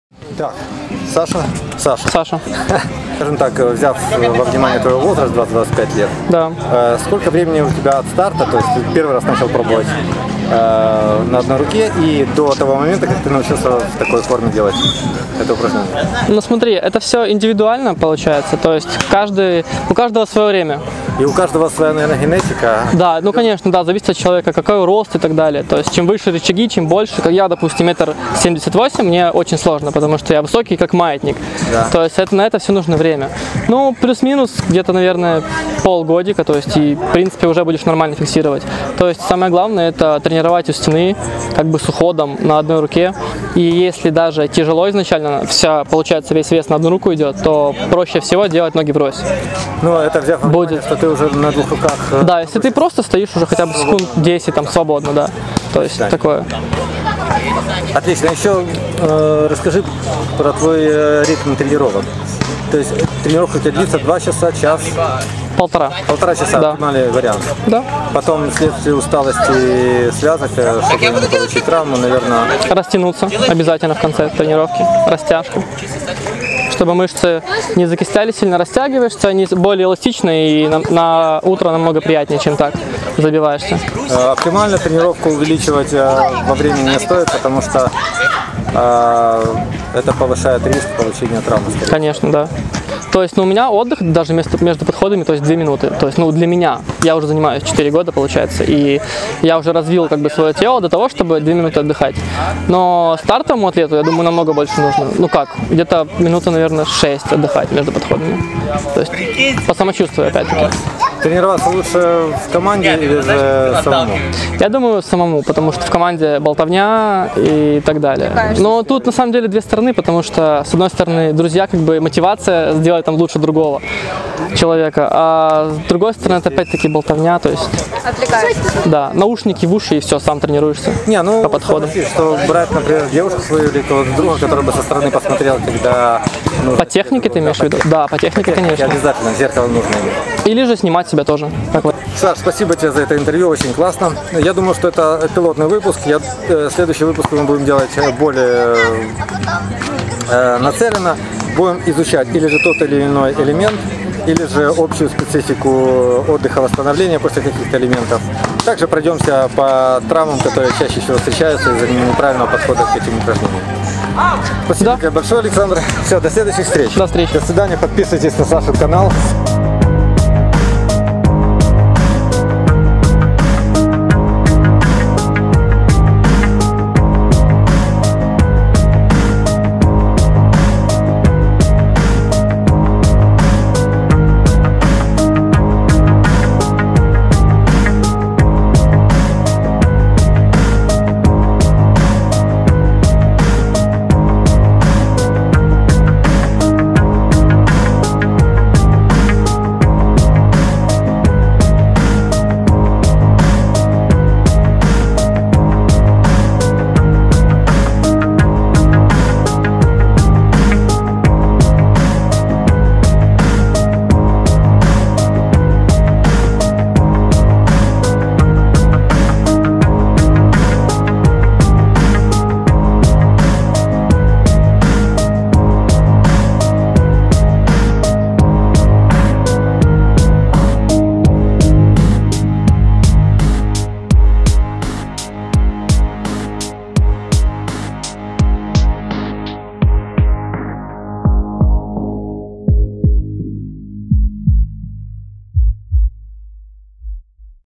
Workout, School #1, Alex vs Alex #2
Contacts of Alexander - контакты Саши

Talking about workout
Alex vs Alex
Первый разговор (особенности тренировок)
First talking (workout features)